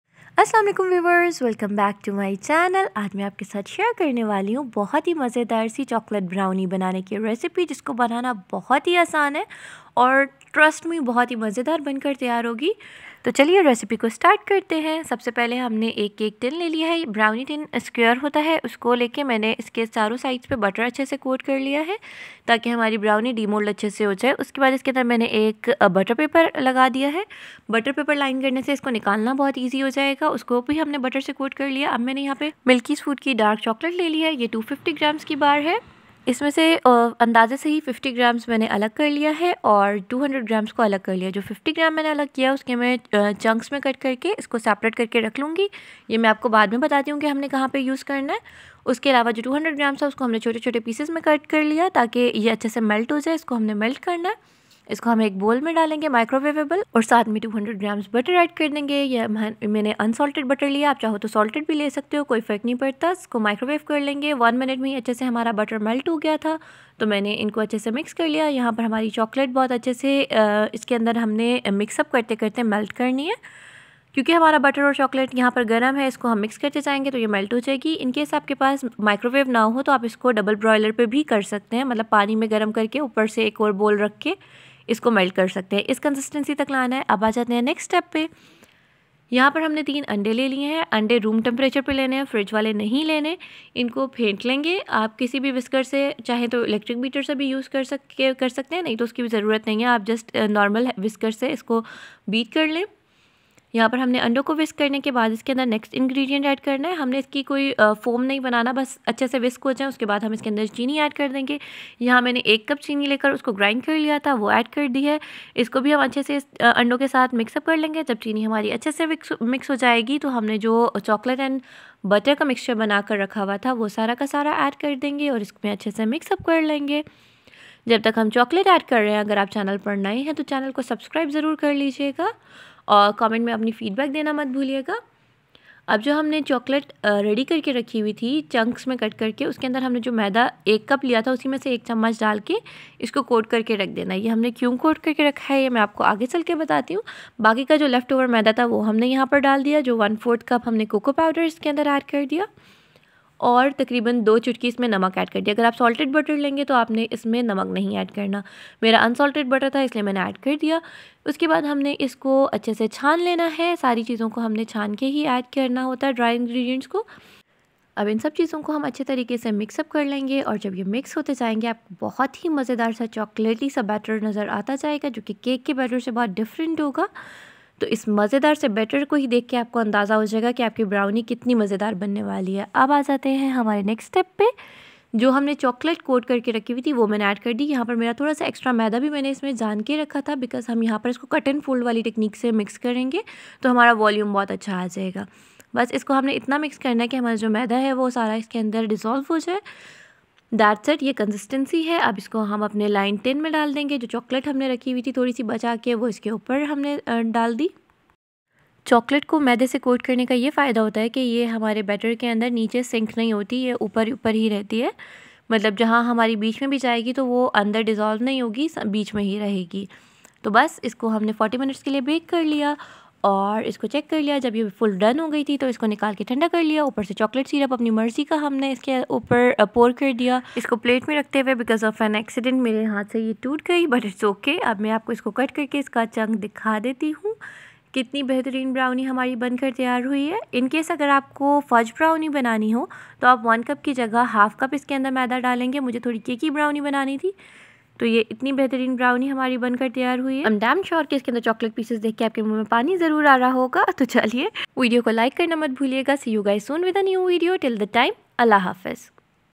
Bakery Style Chocolate Brownie Recipe- Cakey Brownie -Tempting treats by Komal
Bakery Style Chocolate Brownie Recipe- Tempting treats by Komal

•dark chocolate 250 gm
•eggs 3

✓ Line 8×8 tin with butter and butter paper and reserve for later
✓ Microwave 200 gm butter and 200gm dark chocolate
✓ Whisk 3 eggs thoroughly
✓ Add 1 cup castor sugar and whisk until well combined
✓ Add in butter and chocolate mixture and mix
✓ coat 50 gm chocolate chunks with 1 tsp flour
✓ seive flour coco powder and salt in batter
✓ Add in chocolate chunks and mix using cut and fold technique
✓ pour in prepared tin
✓ Bake in pre heated oven for 35-40 mins or until done at 180°
✓ pour some chocolate sauce and enjoy